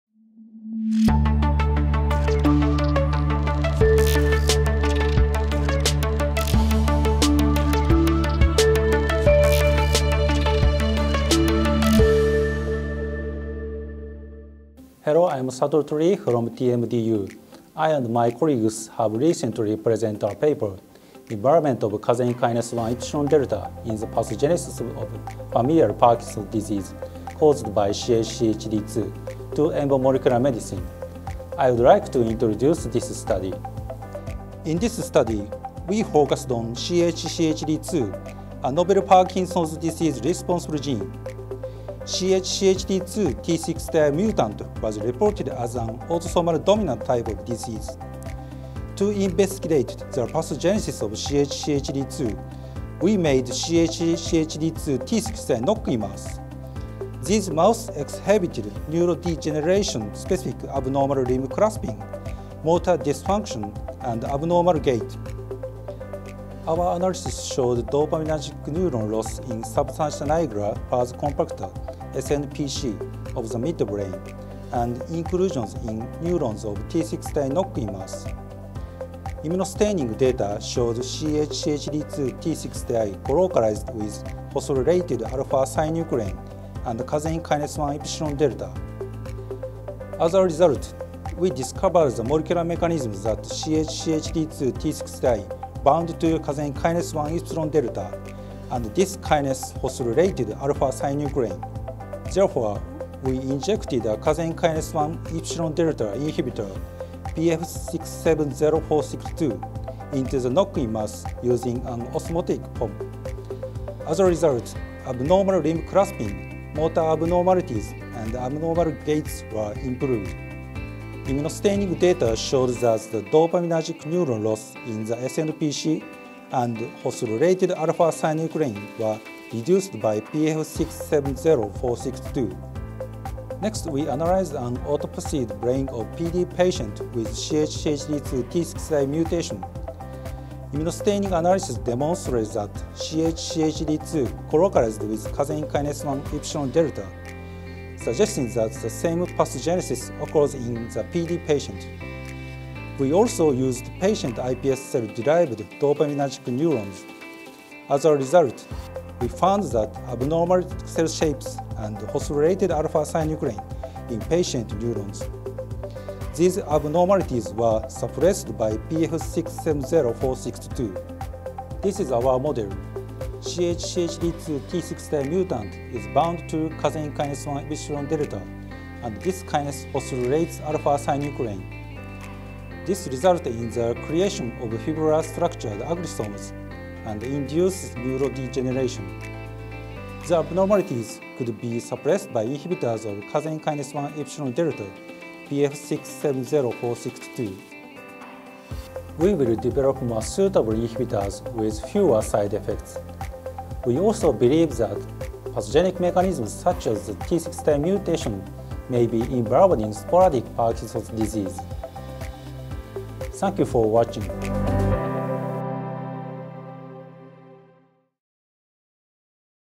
TMDU Research Activities 2023-2024 by Satoru Torii
New insights into the development of Parkinson’s disease in the brain by Satoru Torii, Department of Pathological Cell Biology, Medical Research Institute, Tokyo Medical and Dental University (TMDU)

New insights into the development of Parkinson’s disease in the brain

Paper Information

Involvement of casein kinase 1 epsilon/delta (Csnk1e/d) in the pathogenesis of familial Parkinson's disease caused by CHCHD2

Satoru Torii, Satoko Arakawa, Shigeto Sato, Kei‐ichi Ishikawa, Daisuke Taniguchi, Hajime Tajima Sakurai, Shinya Honda, Yuuichi Hiraoka, Masaya Ono, Wado Akamatsu, Nobutaka Hattori and Shigeomi Shimizu

Publication: EMBO Molecular Medicine
Publication Date: 14 August 2023

Correspondence to

Satoru Torii, Project Associate Professor

Department of Pathological Cell Biology,
Medical Research Institute,
Tokyo Medical and Dental University(TMDU)
E-mail:toripcb(at)tmd.ac.jp

＊Please change (at) in e-mail addresses to @ on sending your e-mail to contact personnels.